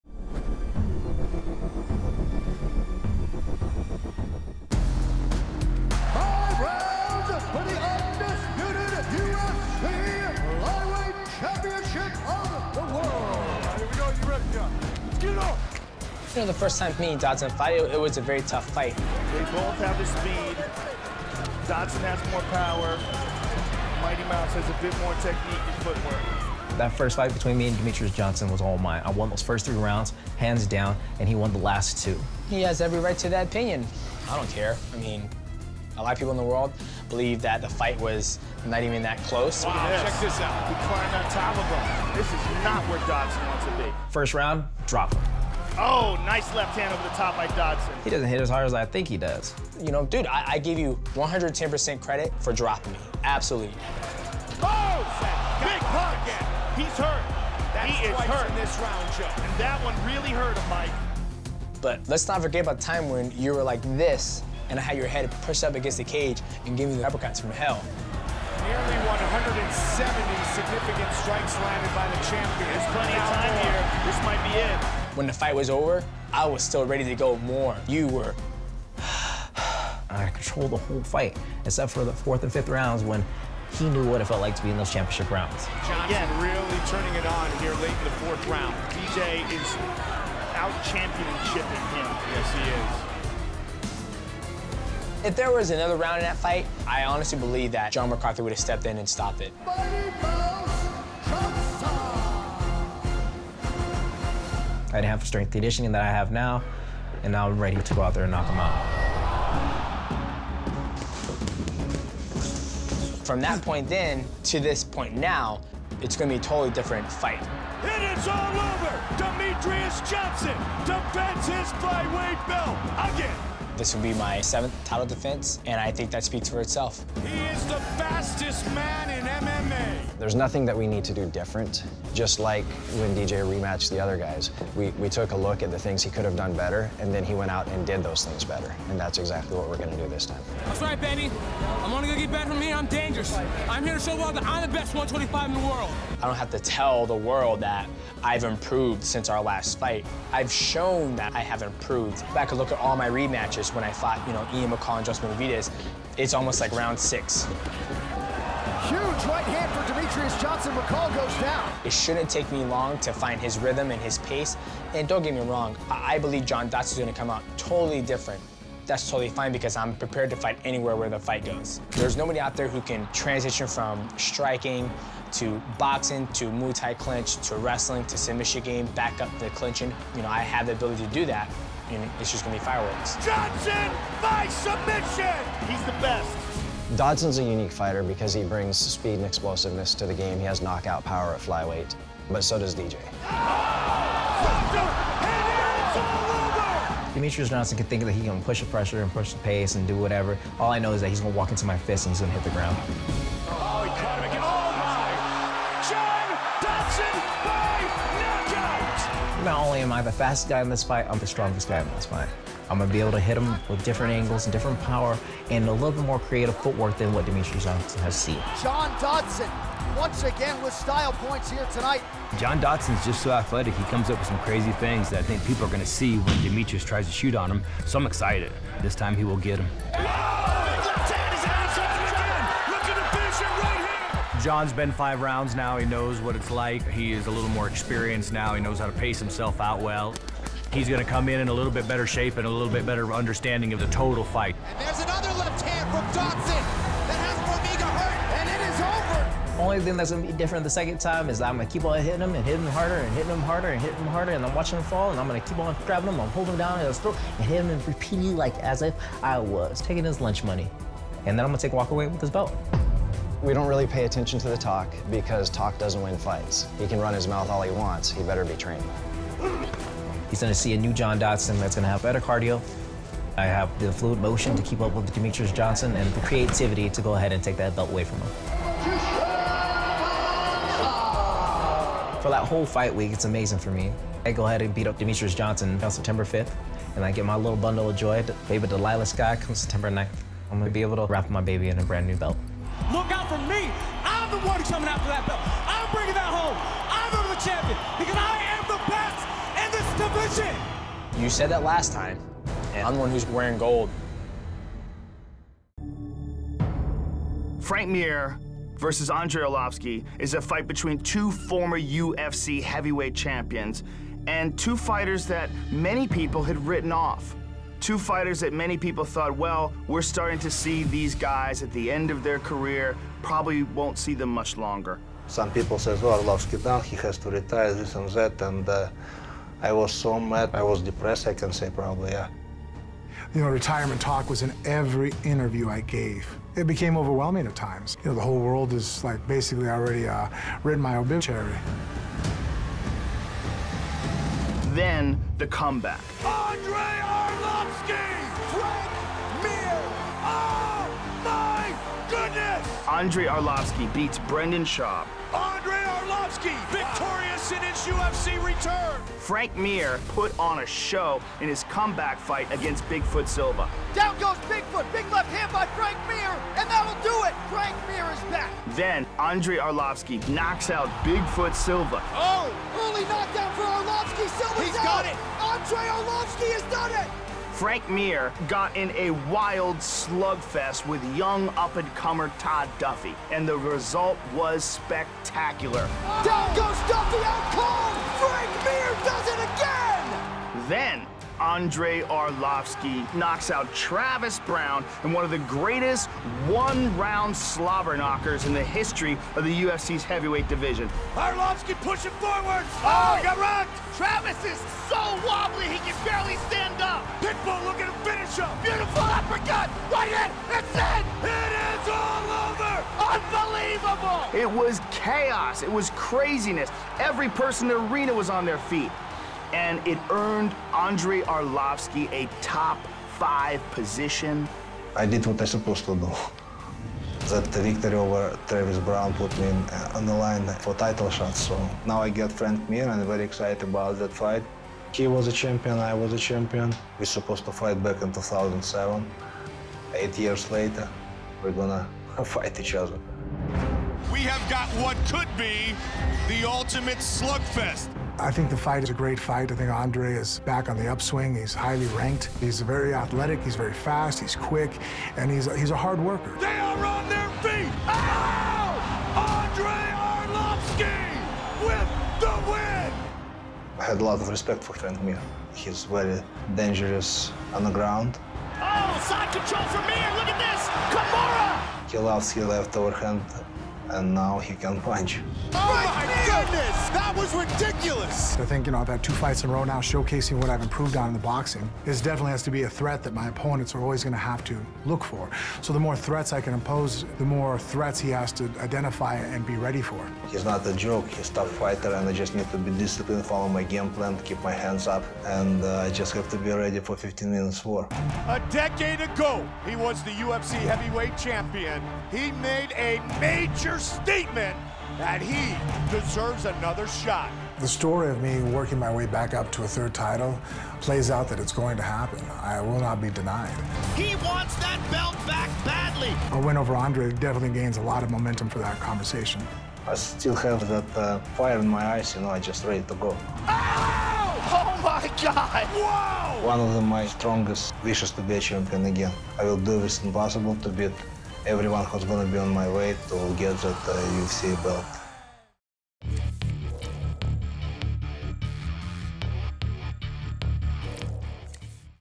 UFC 191: Johnson vs Dodson 2 - Extended Preview
The biggest flyweight fight in the world will be heading to Las Vegas as Demetrious Johnson defends his strap for the seventh time against John Dodson in the main event of UFC 191. The bout is a rematch of a razor-thin unanimous decision in favor of Johnson to retain his title. Now, a little more than 2 years later, they will meet again to settle the score and determine who the true flyweight king is. Plus, top heavyweight contenders Andrei Arlovski and Frank Mir with battle it out in the Octagon to see who will continue to climb the division ranks.